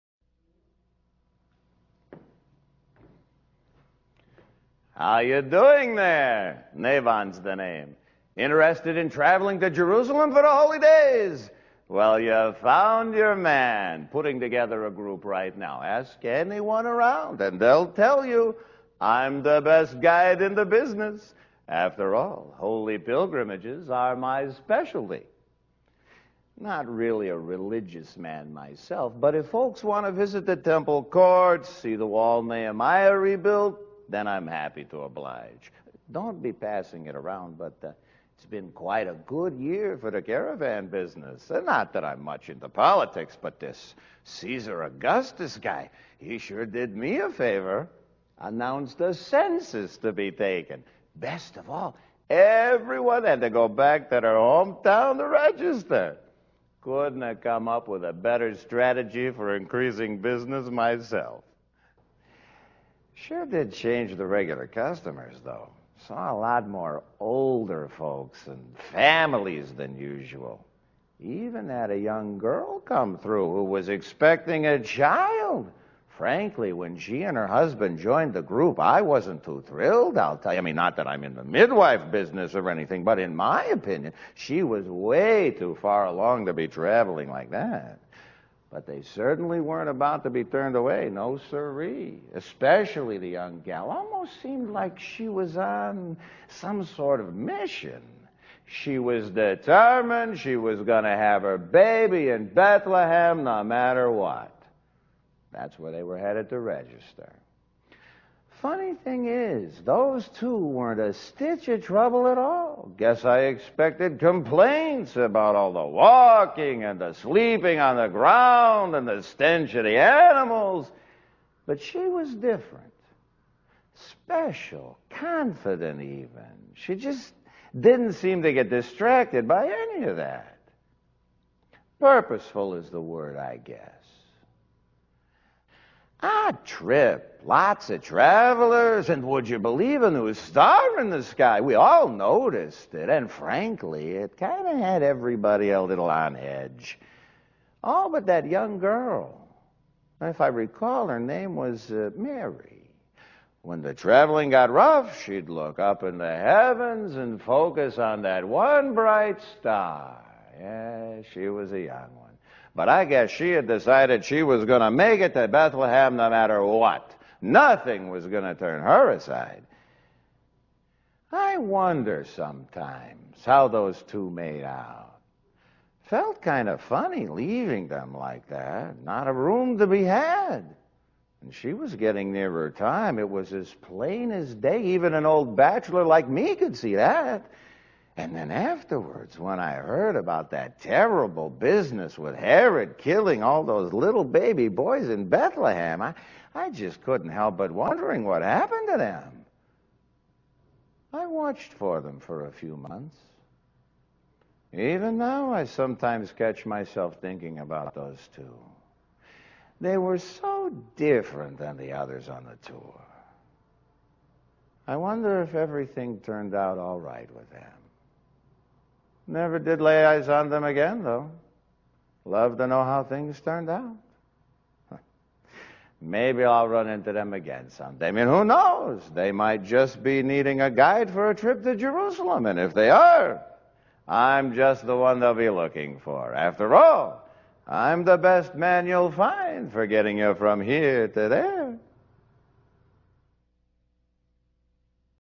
Naybon Caravan preview
In this humorous but thoughtful vignette, guide and caravan-leader Naybon remembers the census taken by Caesar Augustus and the two mysterious travelers on their way to Bethlehem who seemed to have a special purpose.